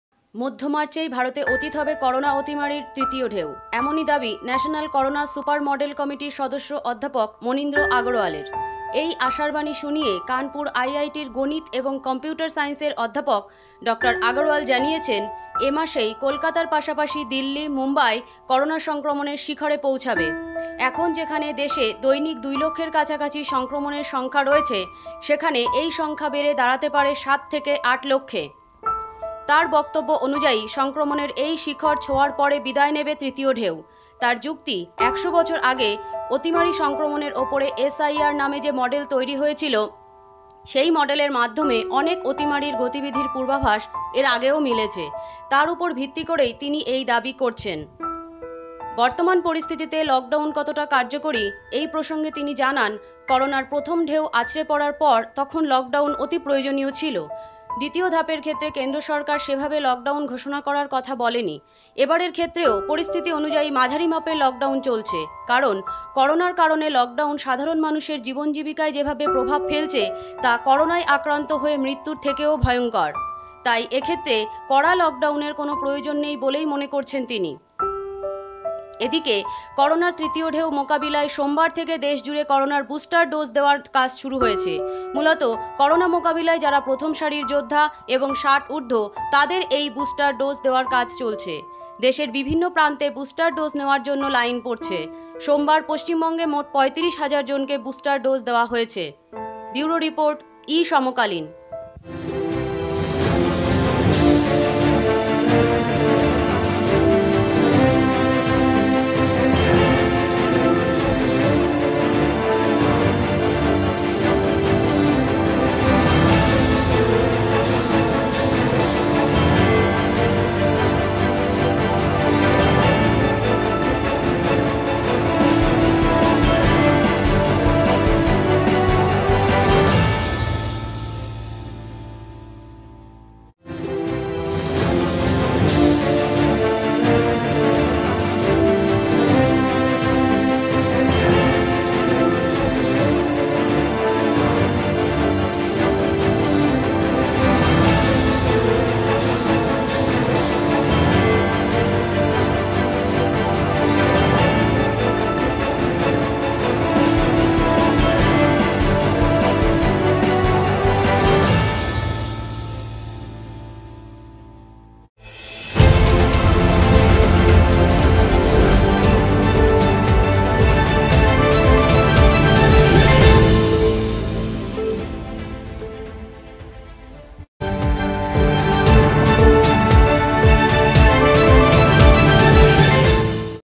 মার্চের মাঝিমাঝি বিদায় করোনার তৃতীয় ‌ঢেউয়ের ? / E Samakalin
The third wave of Corona farewell in mid-March?
মধ্য মার্চেই ভারতে অতীত হবে করোনা অতিমারির তৃতীয় ঢেউ। এমনই দাবি ন্যাশনাল করোনা সুপার মডেল কমিটির সদস্য অধ্যাপক মনীন্দ্র আগরওয়ালের। এই আশারবাণী শুনিয়ে কানপুর আইআইটির গণিত এবং কম্পিউটার সায়েন্সের অধ্যাপক ডক্টর আগরওয়াল জানিয়েছেন, এ মাসেই কলকাতার পাশাপাশি দিল্লি, মুম্বাই করোনা সংক্রমনের শিখরে পৌঁছাবে। এখন যেখানে দেশে দৈনিক দুই লক্ষের কাছাকাছি সংক্রমনের সংখ্যা রয়েছে, সেখানে এই সংখ্যা বেড়ে দাঁড়াতে পারে সাত থেকে আট লক্ষে।
তাঁর বক্তব্য অনুযায়ী সংক্রমনের এই শিখর ছোঁয়ার পরে বিদায় নেবে তৃতীয় ঢেউ। তাঁর যুক্তি, ১০০ বছর আগে অতিমারি সংক্রমণের উপরে 'এস আই আর'‌ নামে যে মডেল তৈরি হয়েছিল, সেই মডেলের মাধ্যমে অনেক অতিমারির গতিবিধির পূর্বাভাস এর আগেও মিলেছে। তার উপর ভিত্তি করেই তিনি এই দাবি করছেন।
বর্তমান পরিস্থিতিতে লকডাউন কতটা কার্যকরী, এই প্রসঙ্গে তিনি জানান, করোনার প্রথম ঢেউ আছড়ে পড়ার পর তখন লকডাউন অতিপ্রয়োজনীয় ছিল। দ্বিতীয় ধাপের ক্ষেত্রে কেন্দ্র সরকার সেভাবে লকডাউন ঘোষণা করার কথা বলেনি। এবারের ক্ষেত্রেও পরিস্থিতি অনুযায়ী মাঝারি মাপের লকডাউন চলছে। কারণ, করোনার কারণে লকডাউন সাধারণ মানুষের জীবন-জীবিকায় যেভাবে প্রভাব ফেলছে, তা করোনায় আক্রান্ত হয়ে মৃত্যুর থেকেও ভয়ংকর। তাই এক্ষেত্রে কড়া লকডাউনের কোনও প্রয়োজন নেই বলেই মনে করছেন তিনি।
এদিকে, করোনার তৃতীয় ঢেউ মোকাবিলায় সোমবার থেকে দেশ জুড়ে করোনার বুস্টার ডোজ দেওয়ার কাজ শুরু হয়েছে। মূলত করোনা মোকাবিলায় যারা প্রথম সারির যোদ্ধা এবং ৬০ উর্দ্ধ, তাঁদের এই বুষ্টার ডোজ দেওয়ার কাজ চলছে। দেশের বিভিন্ন প্রান্তে বুস্টার ডোজ নেওয়ার জন্য লাইন পরছে। সোমবার পশ্চিমবঙ্গে মোট ৩৫ হাজার জনকে বুস্টার ডোজ দেওয়া হয়েছে।